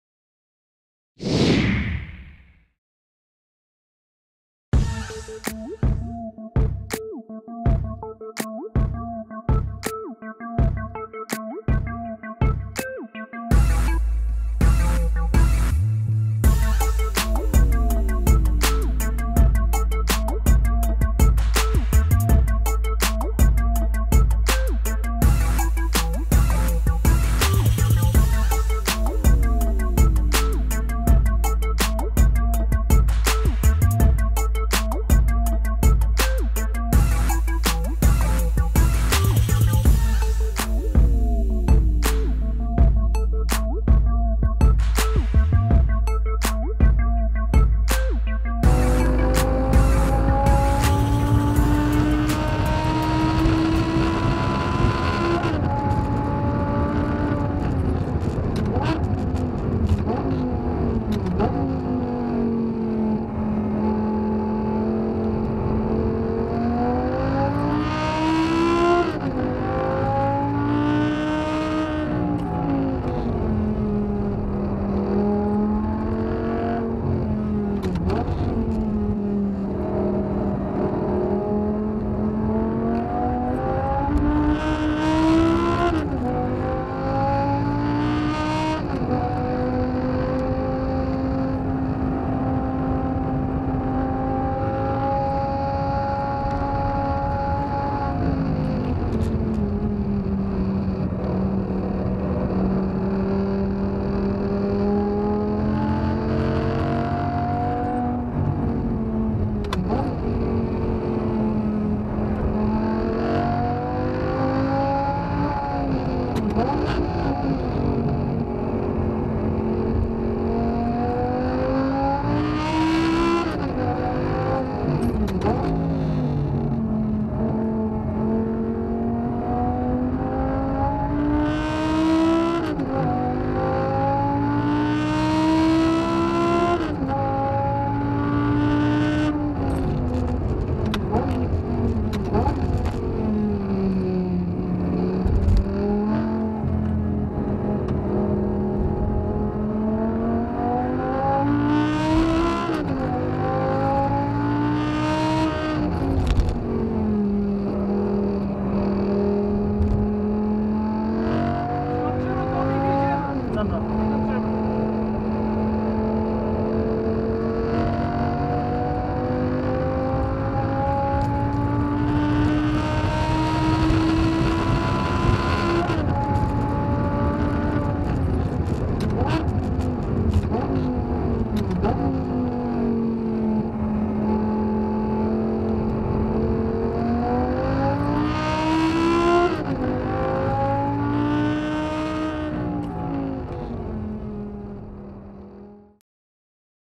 Hotlap Audi R8 V10 600 BHP - Zandvoort racetrack
Hotlap in the first generation Audi R8 V10. Tuned up to 600 BHP and manual! Quicksilver Supersport exhaust. Circuit Zandvoort.